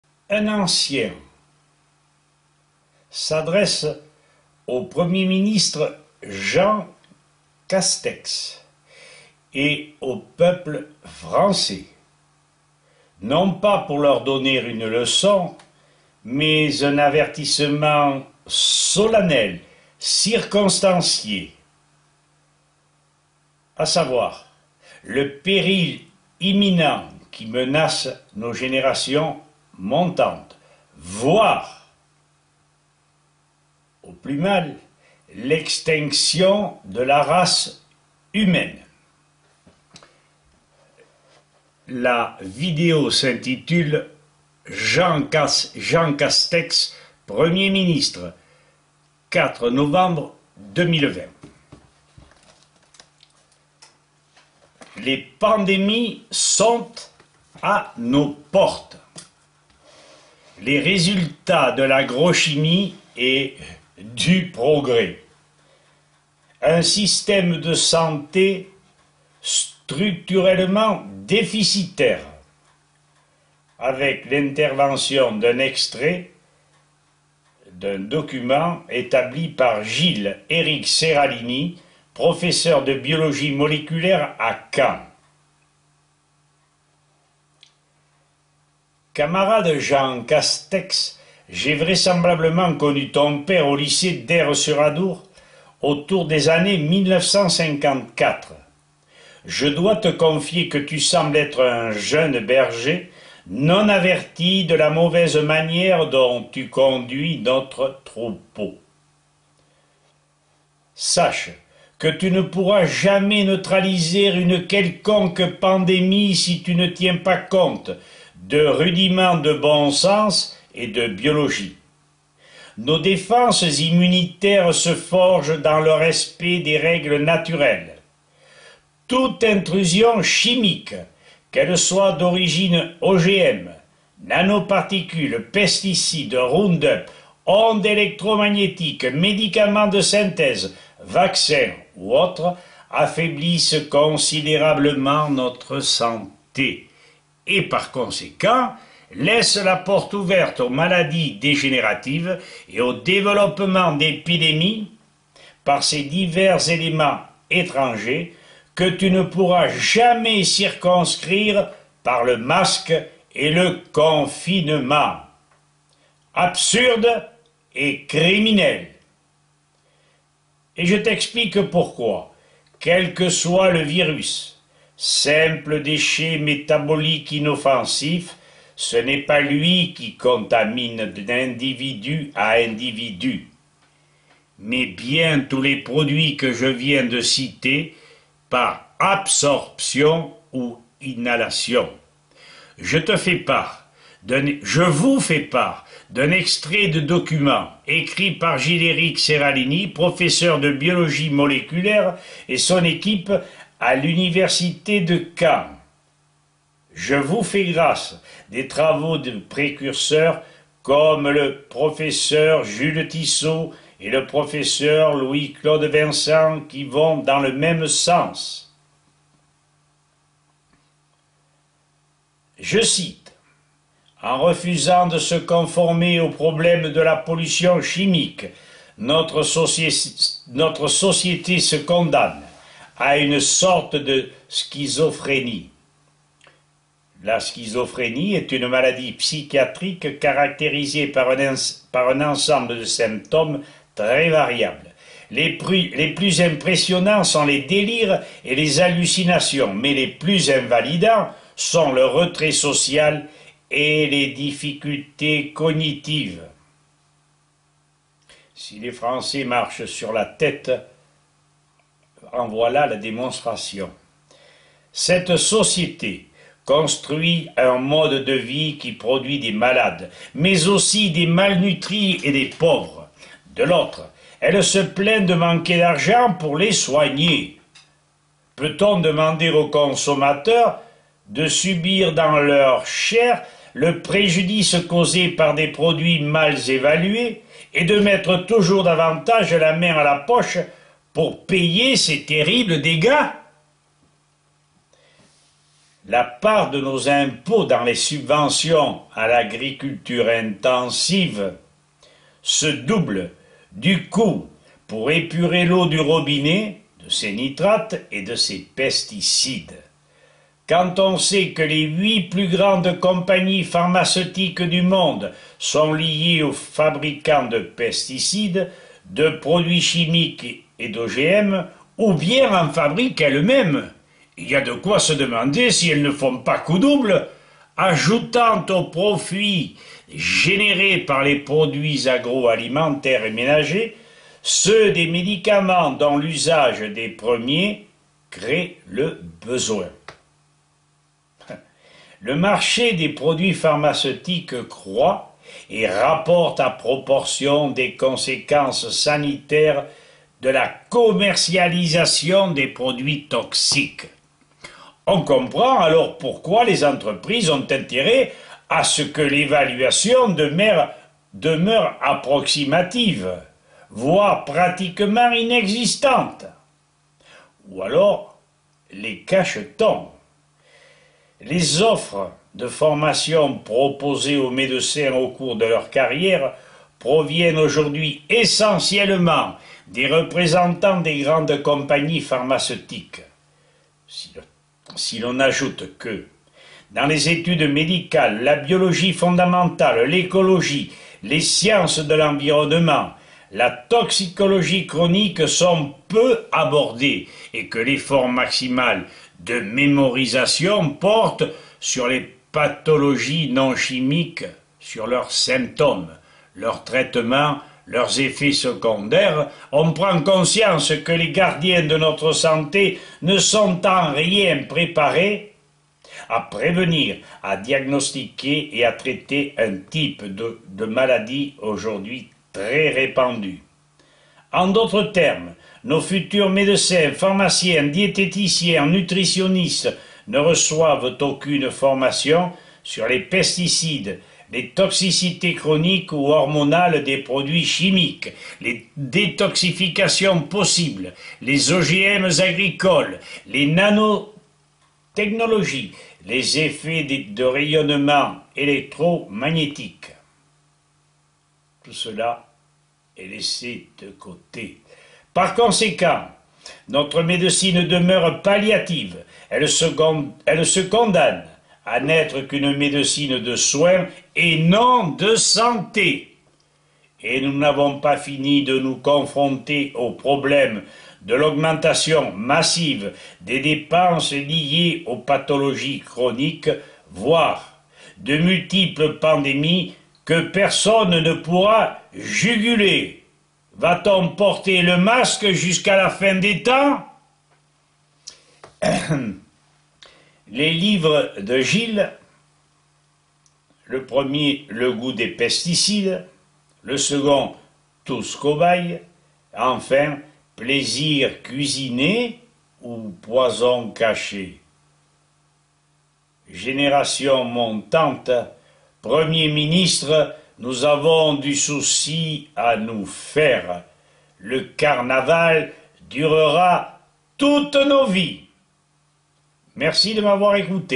Jean Castex 1er Ministre. Masquons nous ?
Que la suggestion de votre ancien et de Séralini soit profitable aux générations montantes. Preuves sont données que nous ne pouvons plus gérer notre Monde de la sorte. Abandons de la chimie sous toutes ses formes.